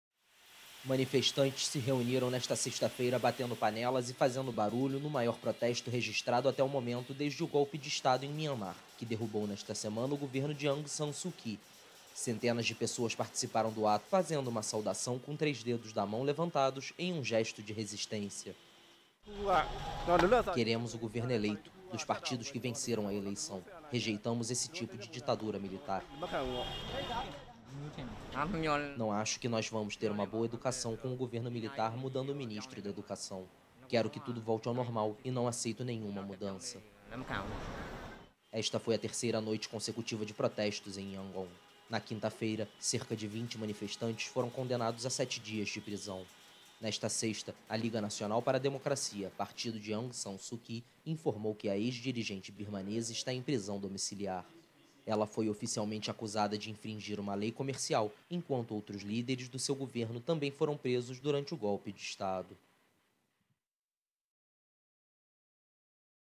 Panelaço e buzinas em protesto contra o golpe de Estado | AFP
Nesta sexta-feira foi realizado o maior protesto contra golpe de Estado em Mianmar, na cidade de Yangon. Professores e estudantes se reuniram em frente a uma universidade batendo panelas e buzinando contra a retirada de Aung San Suu Kyi do poder.